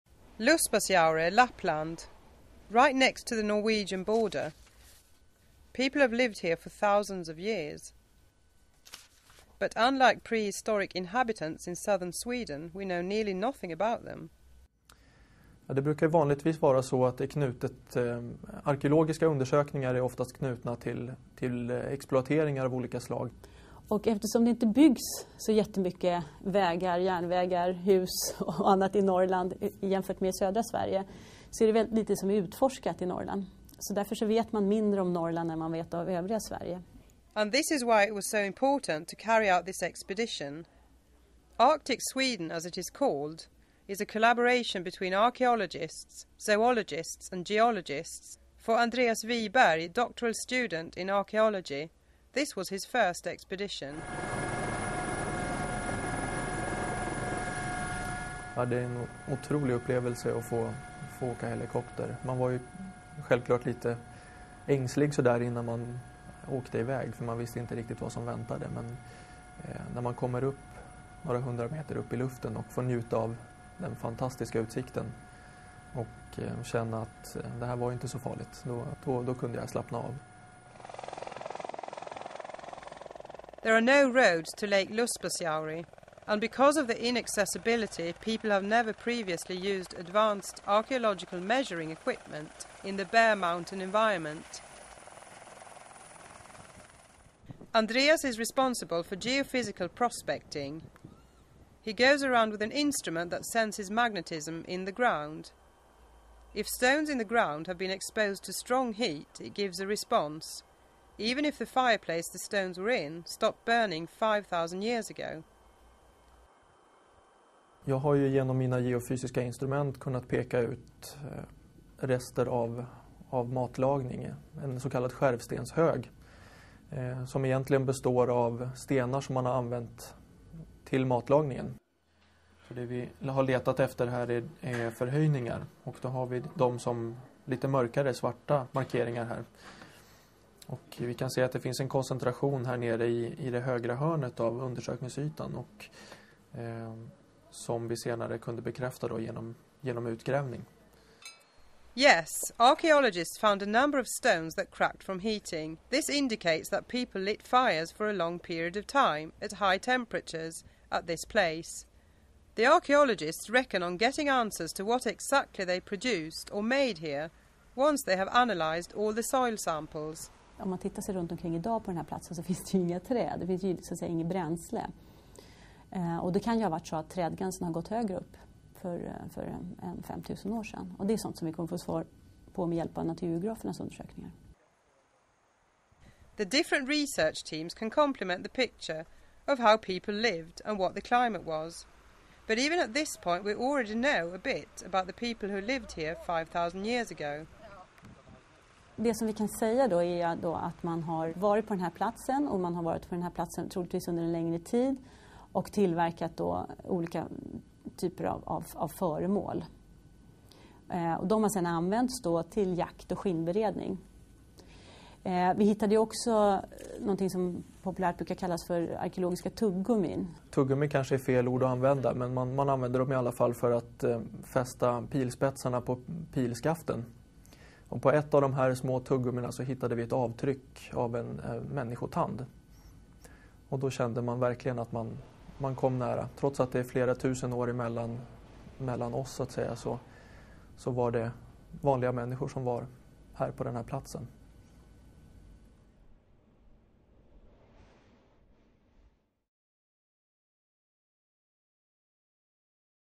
Archaeology in Lapland
How did people live in the far North thousands of years ago? The answer is that we don't really know. Professor of archaeology Kerstin Lidén and archaeological researcher Andreas Viberg have tried to find out more about northern Sweden's past  with interesting results! During their excavations they have even found ancient candy in the earth!